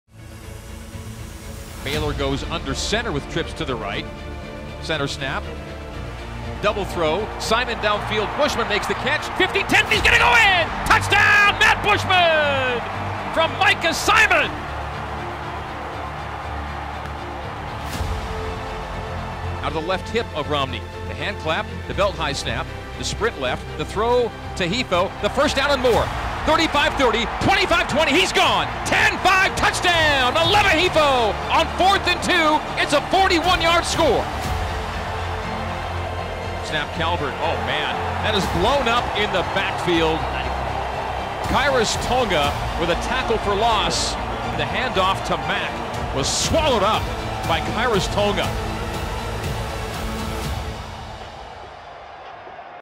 BYU Football - BYU Top Plays - Liberty - November 9, 2019
Top Plays from the BYUFOOTBALL win over Liberty. November 9, 2019